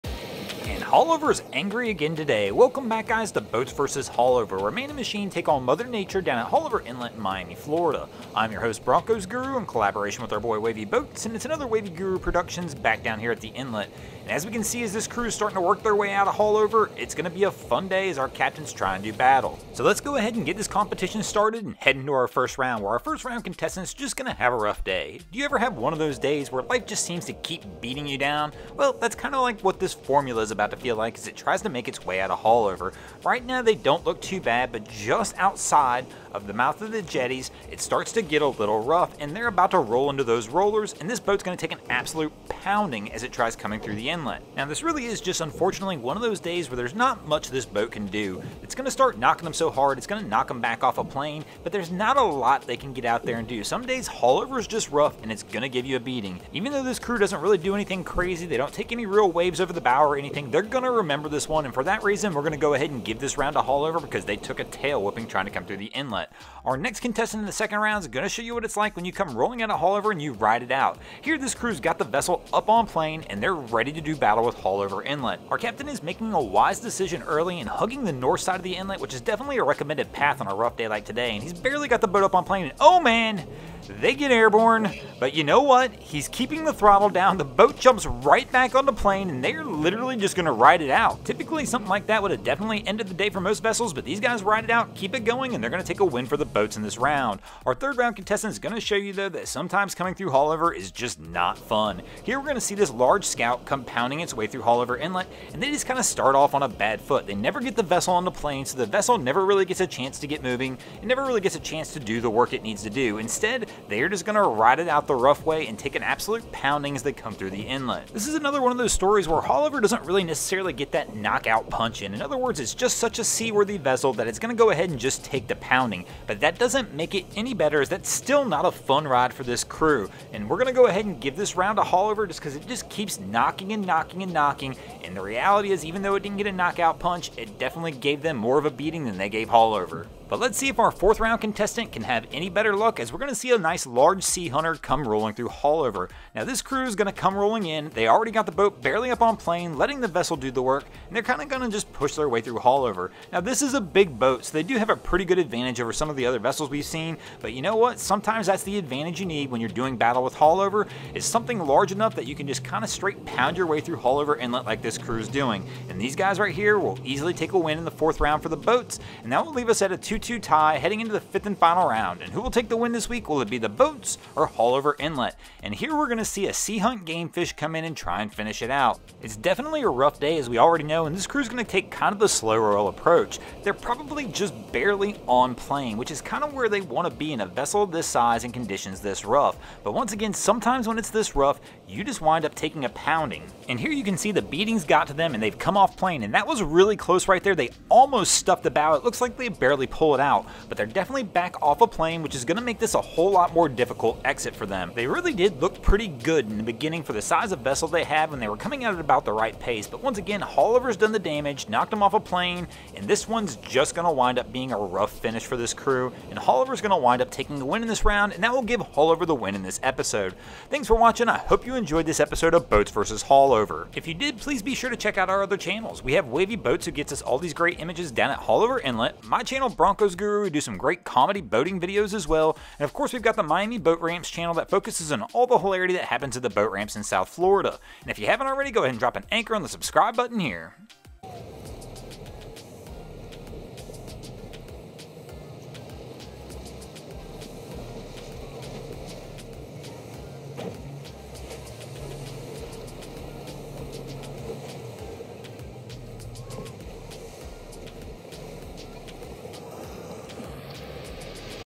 HAULOVER GOING FOR THE KNOCK OUT! | Boats vs Haulover Inlet
Welcome back to another episode where man and machine attempt to do battle with Haulover Inlet!

This episode we are back home at Haulover Inlet. Who will take the win this week between Haulover Inlet and the Boats?

We upload videos twice a week on Saturdays and Wednesdays.